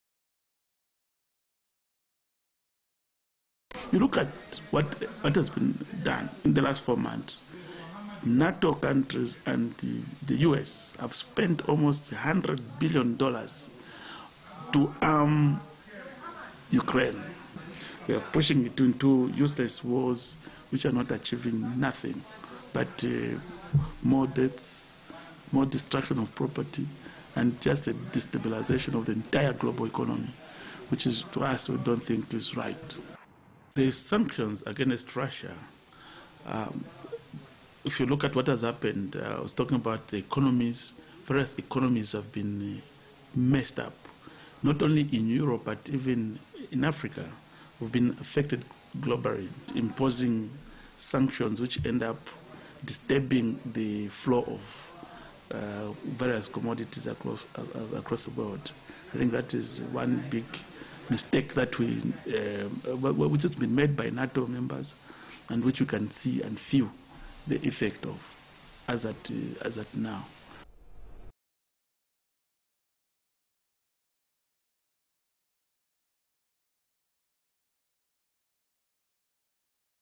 U.S. supply of weapons to Ukraine yields no positive results
The United States should stop supplying Ukraine with weapons, as this is wrong and does not yield any positive results, said Peter Sinkamba, president of the Green Party of Zambia. In the past few months, the United States and North Atlantic Treaty Organization (NATO) countries have provided Ukraine with large quantities of weapons, which has resulted in civilian deaths, damages to property and global instability, said Sinkamba. He noted that the sanctions against Russia by Western countries have brought many negative effects around the world, even in Africa.